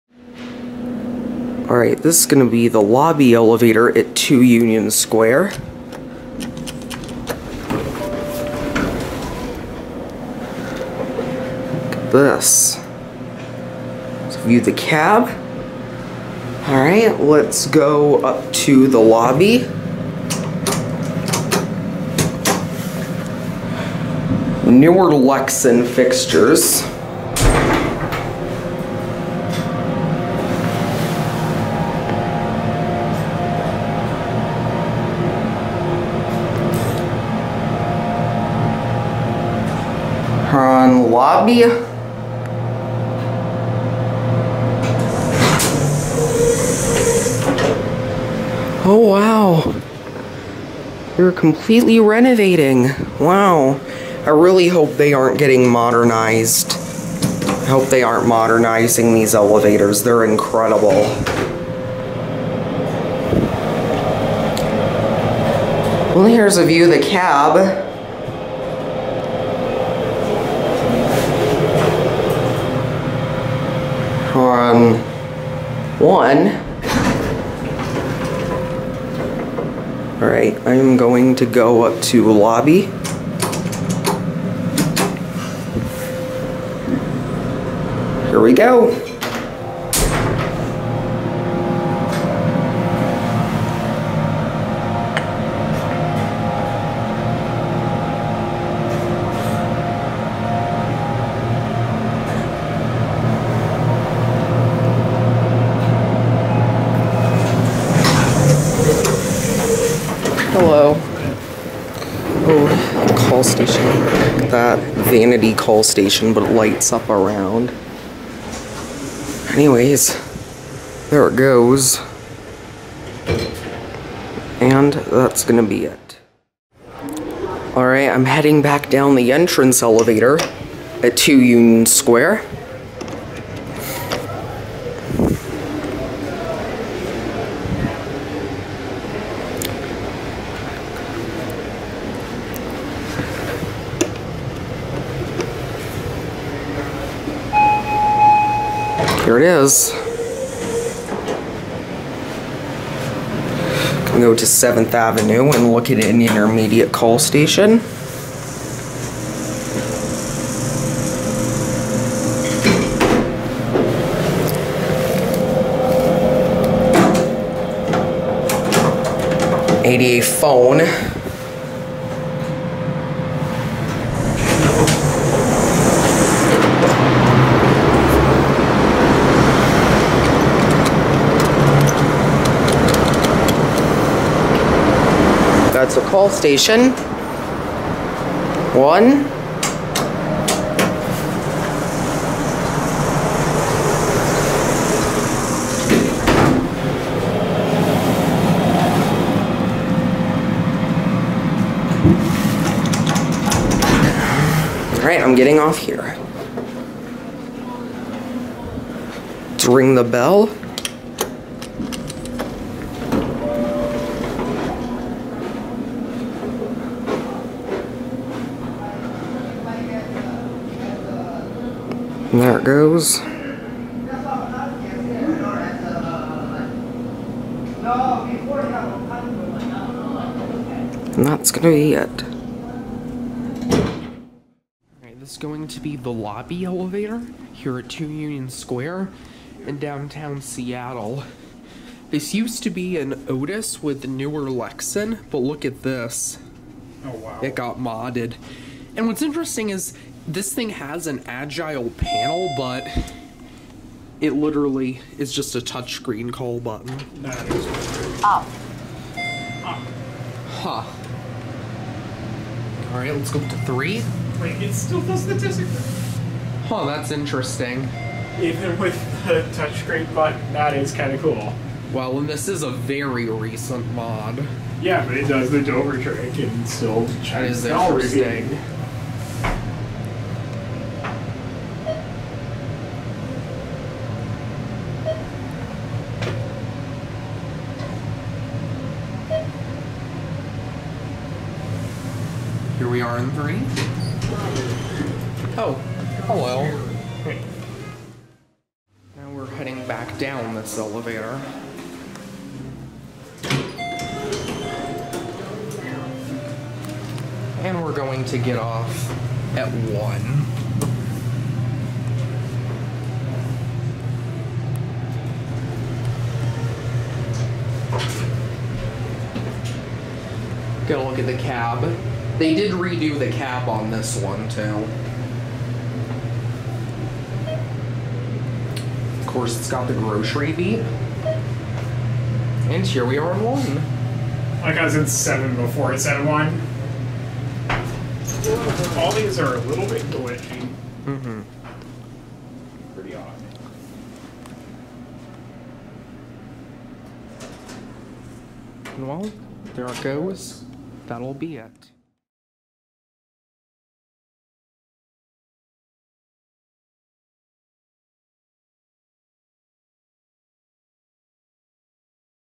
Before & After Modernization: Otis/TKE Hydraulic Lobby Elevator at Two Union Square in Seattle, WA
Filmed on March 22nd, 2018 (Before) & January 3rd, 2023 (After) with ‎@WestCoastElevators .

It's been 5 years since I've been to Seattle and a LOT has changed since then! And pertaining to elevators, since my last visit to Seattle, ALL of the elevators at Two Union Square have now been modernized by TKE Elevator (formerly Thyssenkrupp). Originally Otis with Newer Lexan fixtures, the Main Elevators (which were Elevonic 401s) that serve the Tower are now TK AGILE while this elevator and the Parking Elevators now have MAD BS COPs on this inside, but have AGILE Panels for the call stations! As to why they did this, I have NO IDEA, but it's definitely interesting. It even still has the "Dover trick" despite how recent this mod is and the fact it uses a Touchscreen Call Station which usually changes how the logic behaves.
This modernization isn't too bad, while I do much prefer the original Newer White Lexan fixtures, Series 2 style indicators, Motor and Vanity Call Stations the original elevator had, the New Mod here is by no means bad and it's definitely quite unique! Now, that all being said, given this is just the lobby elevator, I could care less about this one being modded. My feelings about the Main Elevators here being modded now are MUCH MUCH Stronger than how I feel about this particular one being modded even though I do find the mod here to be good albeit very unfortunate regardless to which elevator you're referring to.
With that being said however, I hope you enjoy this video, featuring NEVER BEFORE RELEASED Footage of this elevator before the Mod as well as some Fresh Footage of this Elevator after it's rather unique and interesting albeit still unfortunate mod! And stay tuned for my video of the Main Elevators here, I know you guys are both wanting to see them and are also wondering what my thoughts on those are having ridden them now!

Installed in 1989. (Otis 211 with Newer White Lexan fixtures)

Modernized in Circa 2020 or 2021. (TKE Elevator with New Motor, New Cab, New Controller, New Call Stations, New Lanterns, MAD BS Moon fixtures, etc. Only the outer doors were left original.)